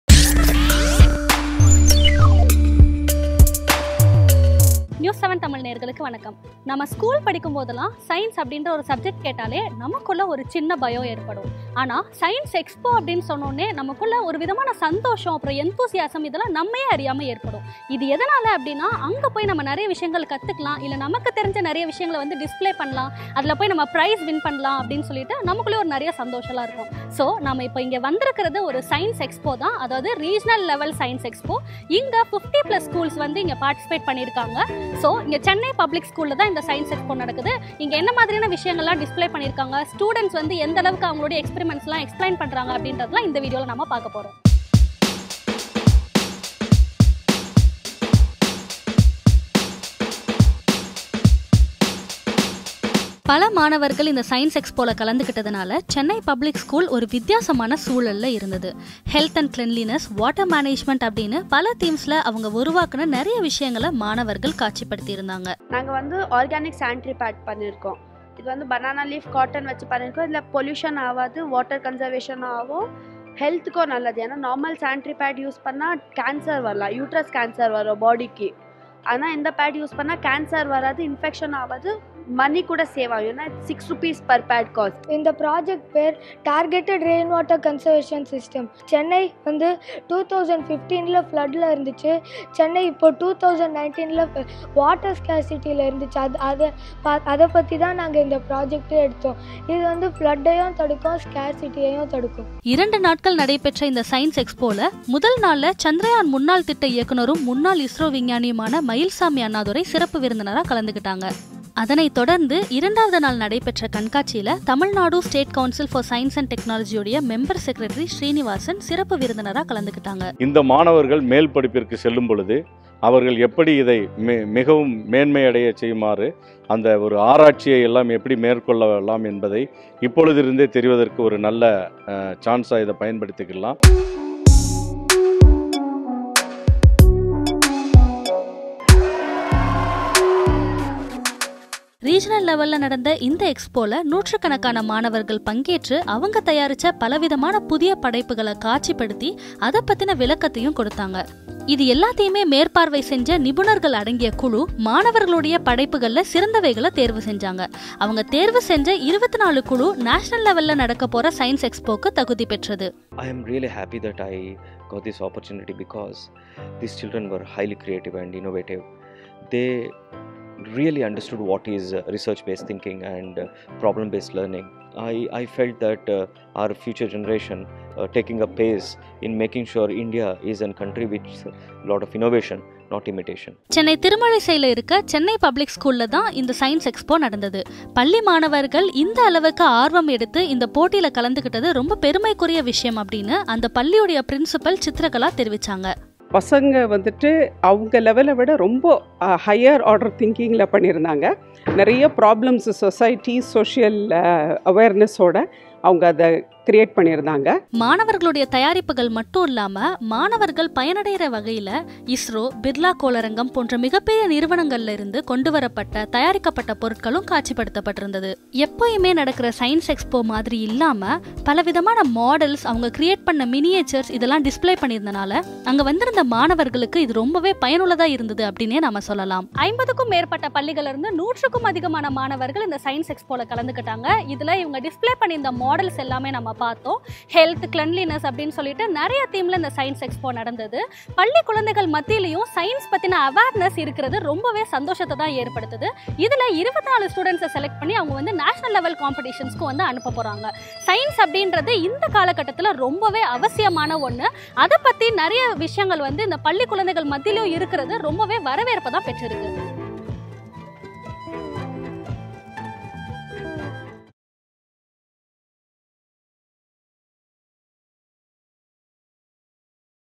வியக்கவைக்கும் இளம் விஞ்ஞானிகளின் படைப்புகள்...! | Science Expo - 2019
NEWS7 TAMIL

வியக்கவைக்கும் இளம் விஞ்ஞானிகளின் படைப்புகள்...!

Regional Level Science Exhibition where more than 50 Schools participated and exhibited the models created by them.